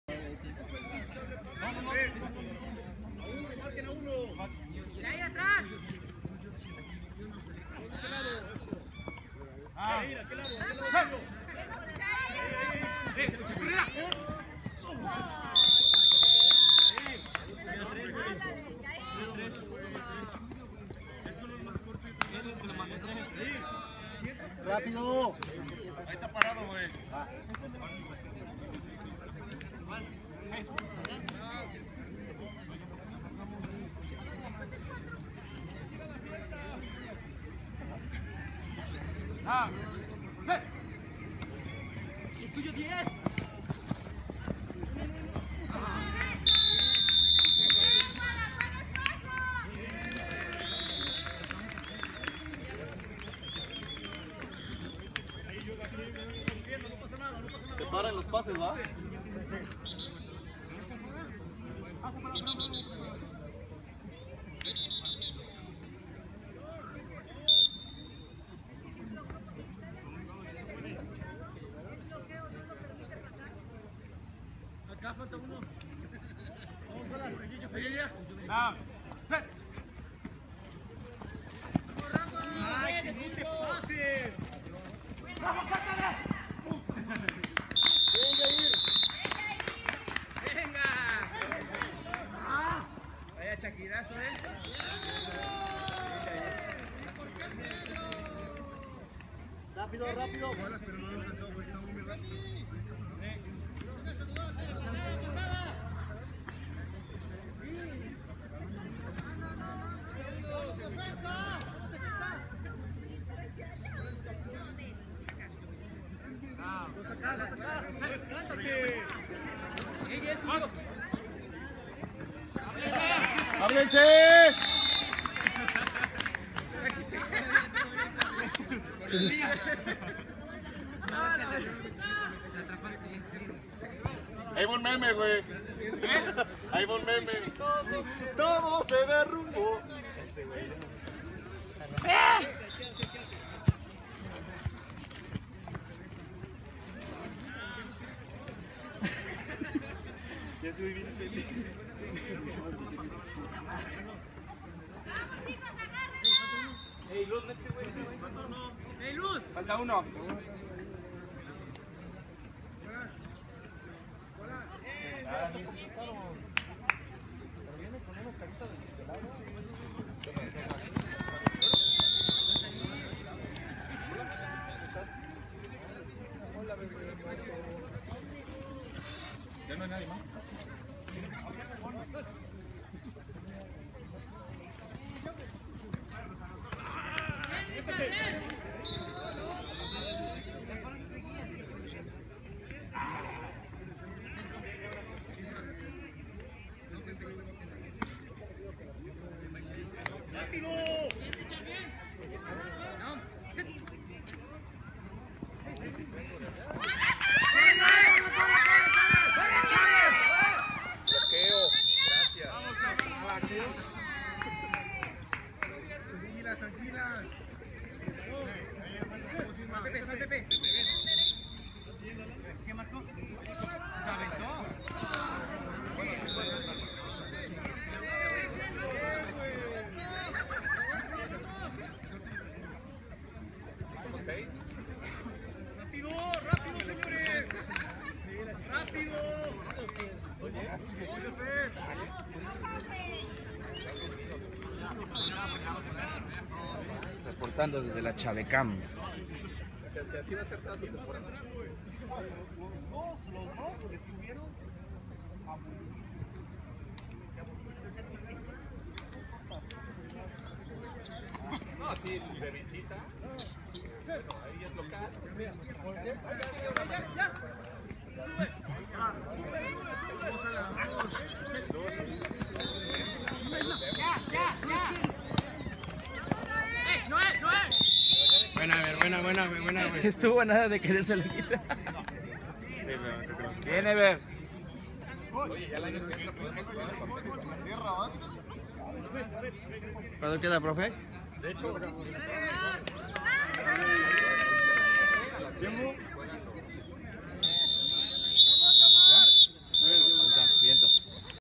Pericos Vs. Leones CBTIS 40-2 Tocho Bandera Xalapa (2/5)
2da parte del partido de Pericos Vs. Leones del CBTIs, marcador final 40-2 a favor de Pericos